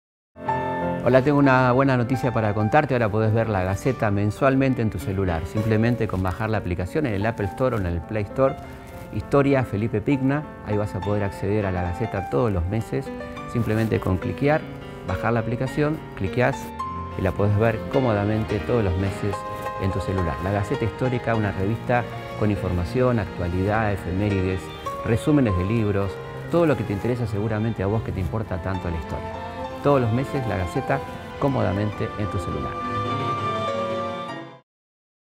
App Felipe Pigna - Gaceta Histórica
Visitá nuestra GACETA HISTÓRICA en tu tablet o celular.
Como siempre, además de los artículos, también accedé a las obras de teatro en cartel, películas, muestras en diversos museos, efemérides, lanzamiento de libros y noticias relacionados con la historia. Pueden descargarse desde AppleStore o GooglePlay/PlayStore ¡Que la disfruten!